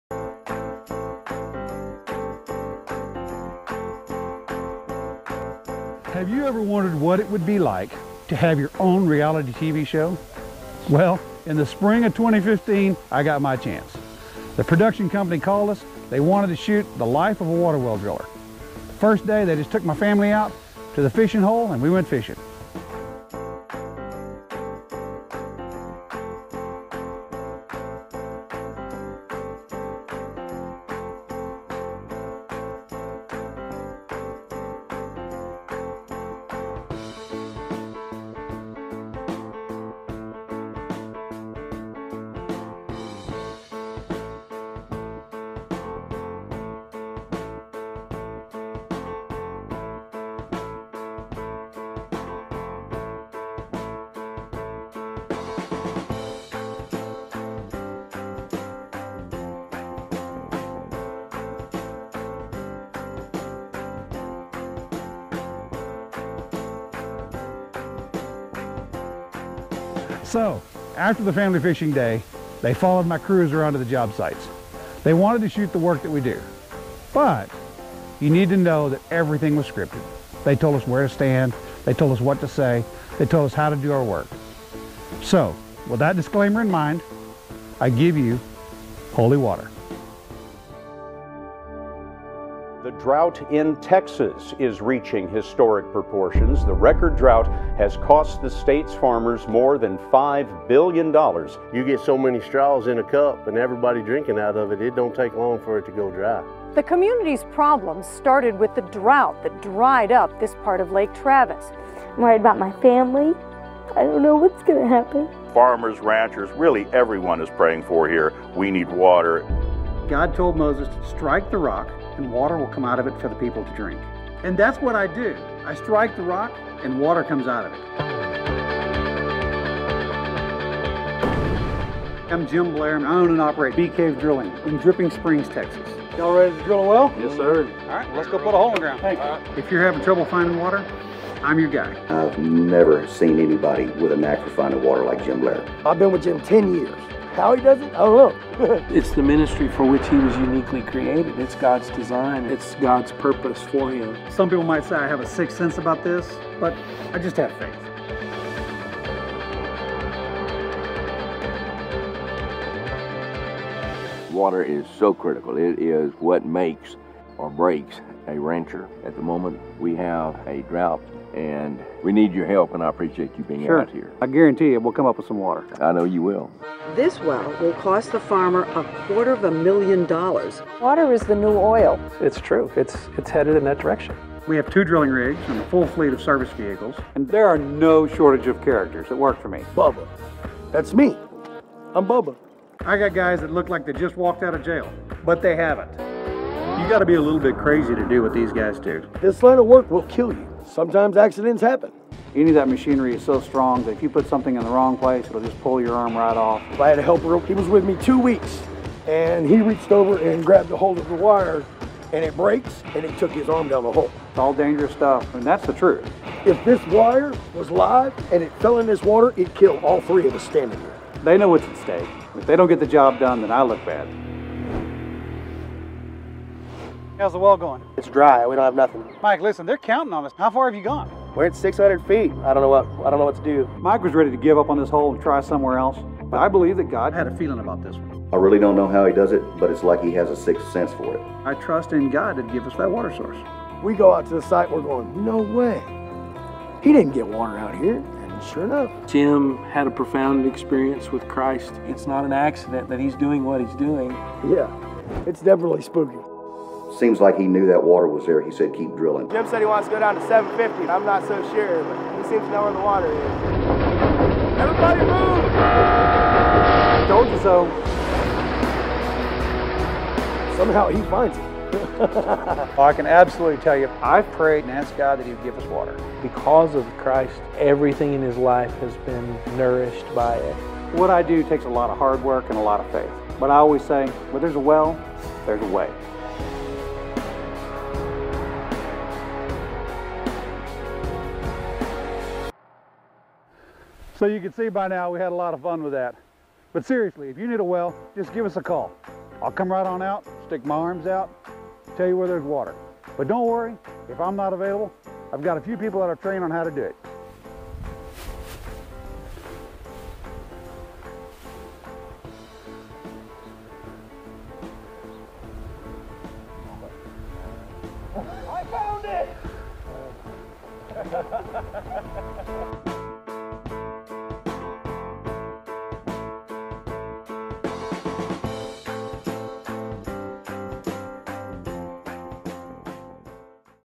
Bee Cave Drilling Reality Show
Have you ever wondered what it would be like to be on a reality tv show? Well, recently our President Jim Blair was approached by a production company and here's what happened!